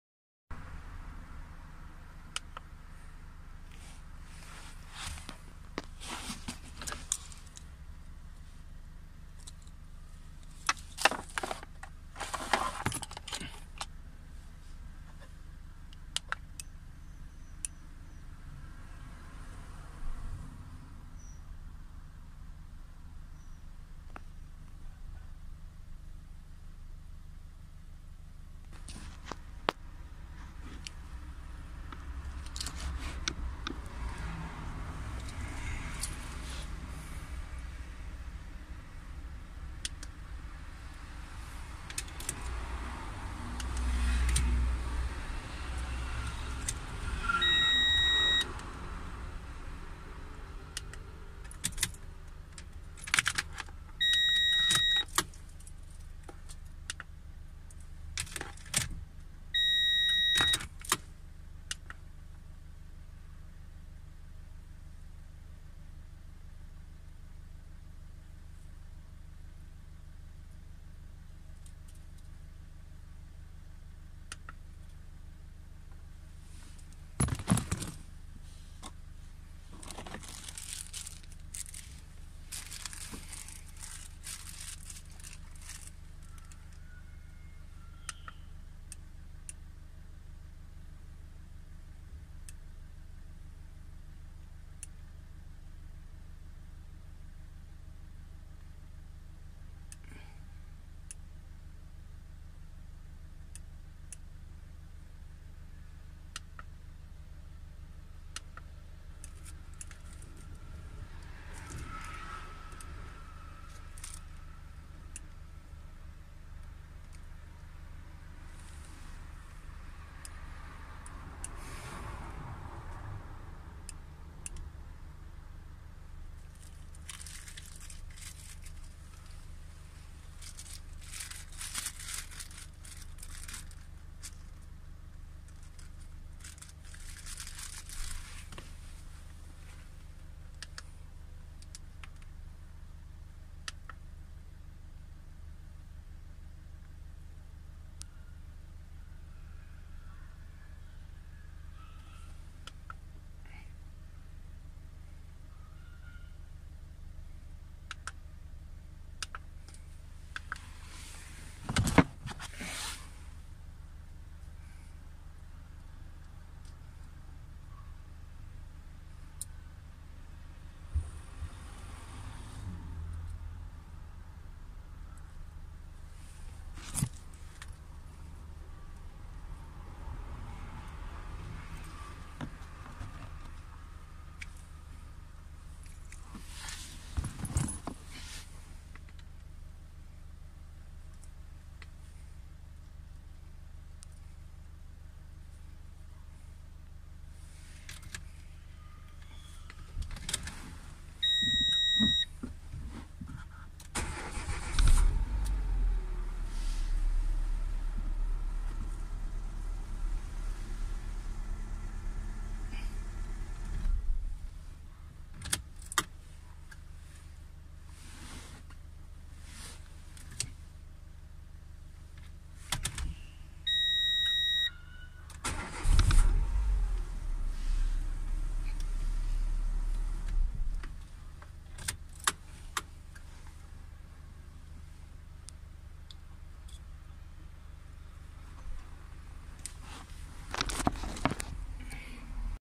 Programmation clé démarrage Renault.. Programming key renault.. Lonsdor kh100
Decoder clé Renault id46
Decoder key renault
Chip id46
Maintenance multi service auto Toulon France..
1 euros symbolique pour l'aide de maintien de la chaîne youtube..